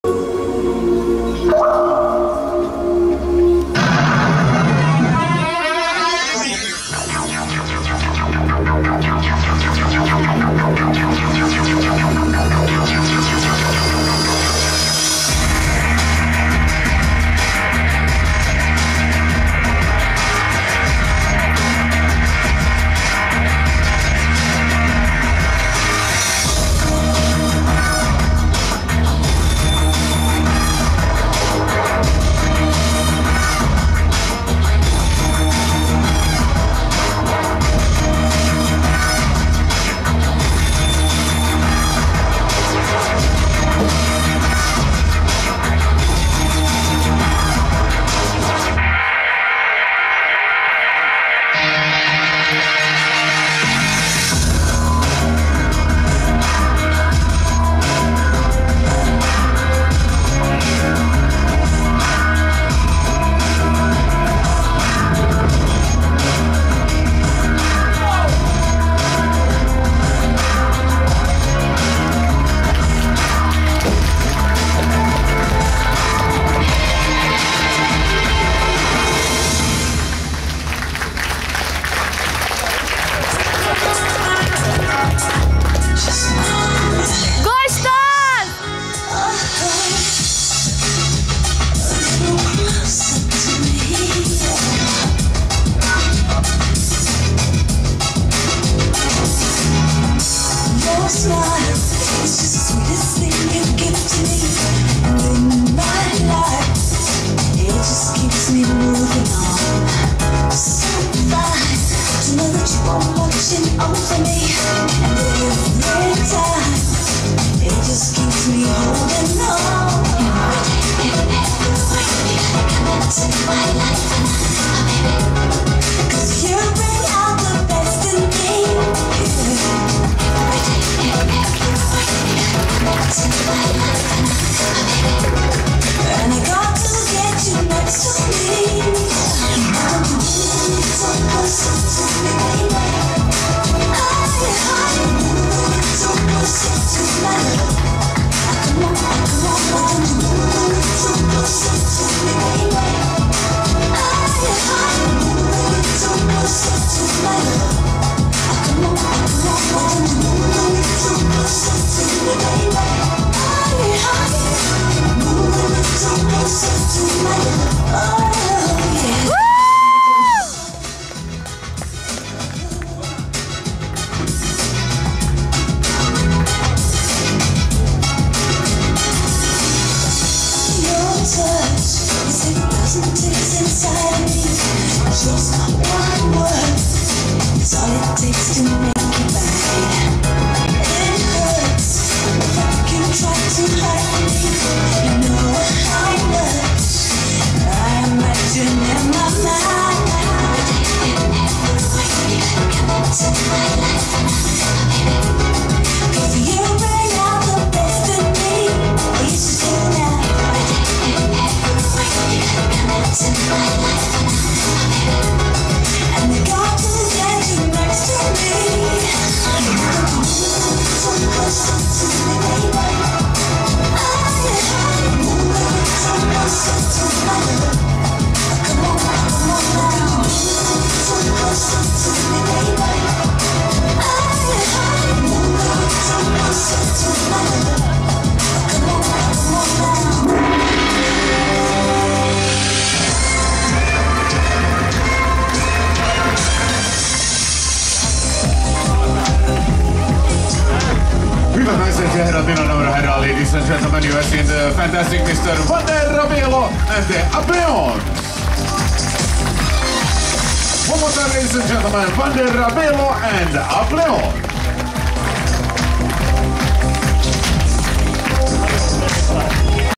Wander Rabelo (Levitation)
incredible appearance of a woman, followed by a levitation with a final surpriendente...